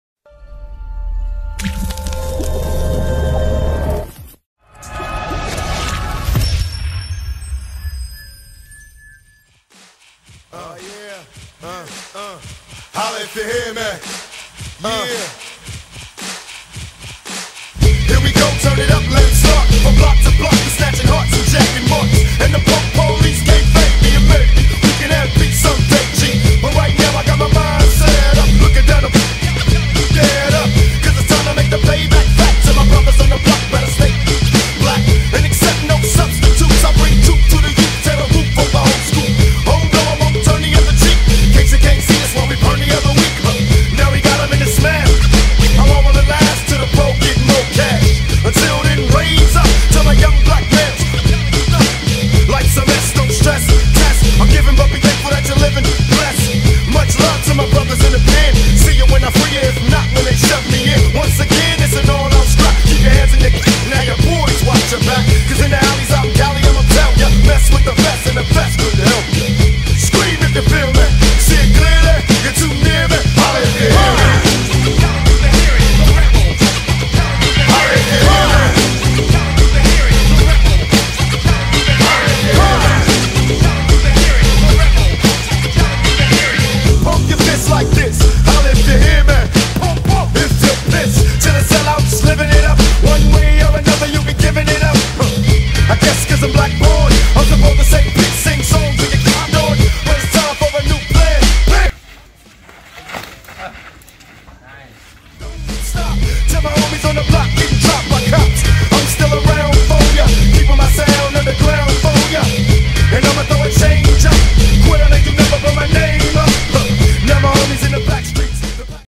Full Contact Stick Fighting | Pekiti Tirsia Kali
Pekiti Tirsia Kali World Federation Mastery Camp 2019. Part of the Mandala Rank Promotion involved three days of evaluation by Tuhon Rommel Tortal. The process ended with the last day of stick sparring at the end of the Mastery Camp 2019.